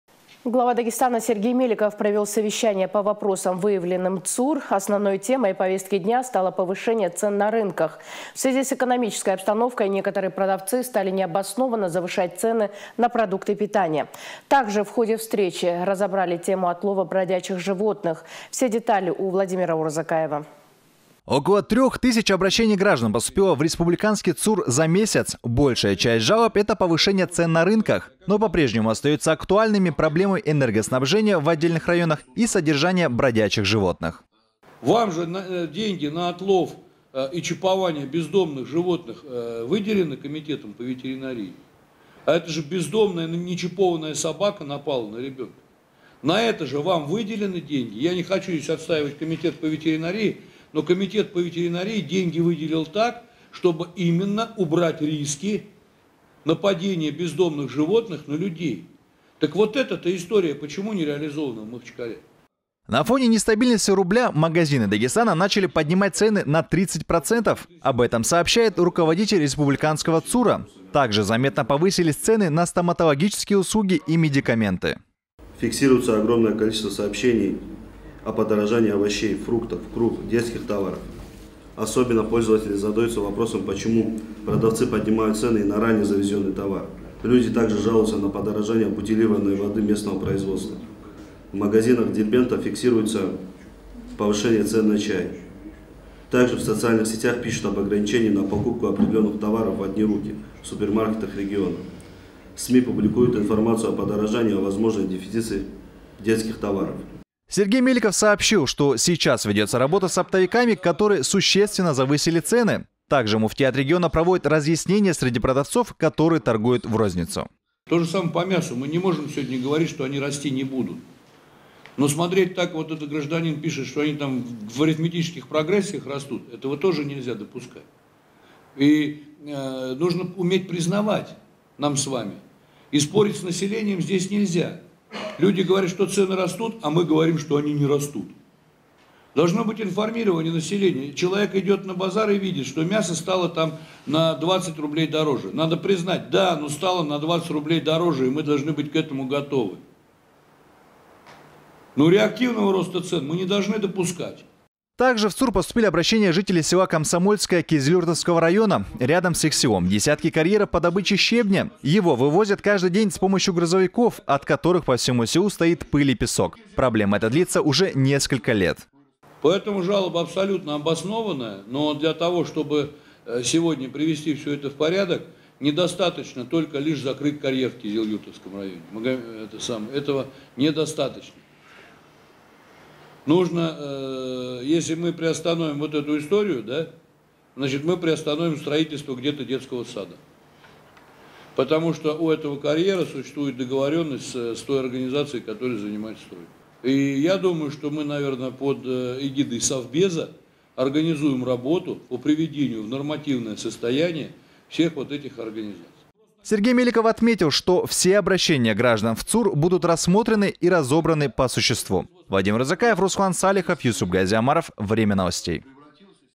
Сергей Меликов провел совещание по вопросам выявленным ЦУР
Глава Дагестана Сергей Меликов провел совещание по вопросам, выявленным ЦУР. Основной темой повестки дня стало повышение цен на рынках. В связи с экономической обстановкой некоторые продавцы стали необоснованно завышать цены на продукты питания. Также в ходе встречи разобрали тему отлова бродячих животных.